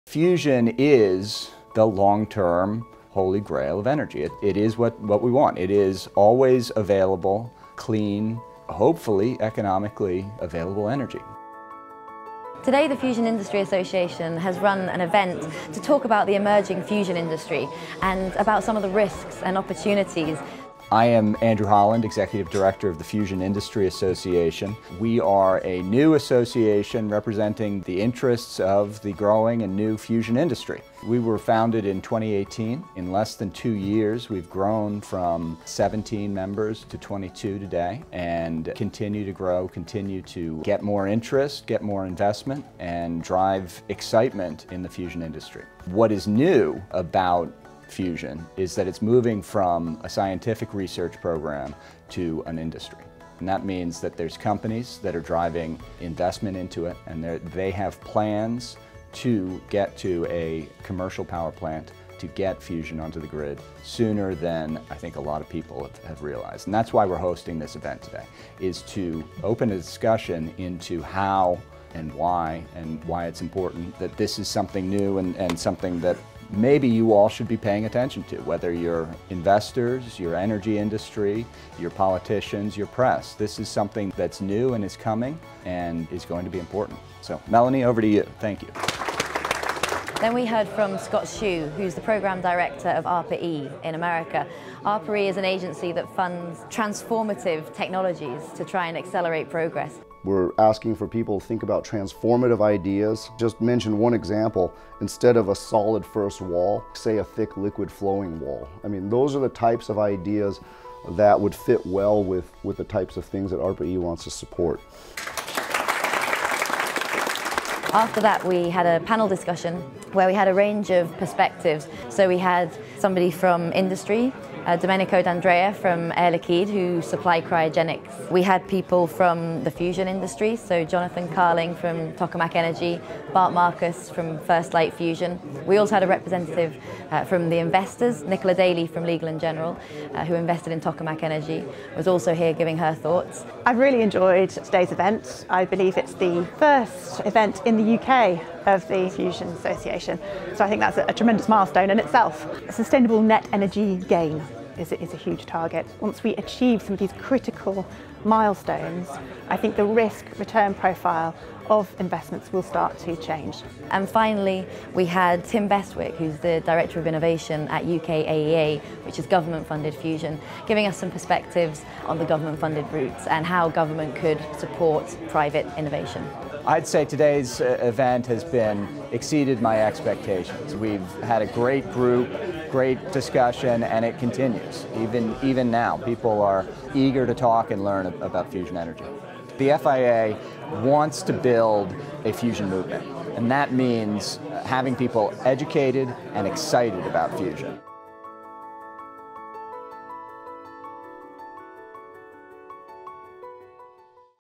Emergence of the fusion industry panel event, London
Fusion, the energy of the stars, offers the possibility of clean, safe and abundant energy on Earth.  And it's coming.

In March 2020 the Fusion Industry Association ran an event in London, UK, to talk about the emerging fusion industry and some of the associated opportunities and risks for businesses and investors.

In this video, hear from Executive Director Andrew Holland about the growing fusion industry and what is new in the field, and from UK Director Melanie Windridge about the contributions from our panellists.  Nicola Daly, Senior Investment Analyst at Legal & General, gives her thoughts on the milestones that need to be achieved for the risk-return profile for investments in fusion to shift.

Contributors:
Scott Hsu, Program Director, ARPA-E
Nicola Daly, Senior Investment Analyst, Legal & General
Jonathan Carling, CEO, Tokamak Energy
Bart Markus, Chairman, First Light Fusion
Domenico d'Andrea, Senior Business Developer, Fusion, Air Liquide
Tim Bestwick, Director of Business & Innovation, UKAEA Culham Centre for Fusion Energy.

Video production funded by Tokamak Energy, member company of the Fusion Industry Association.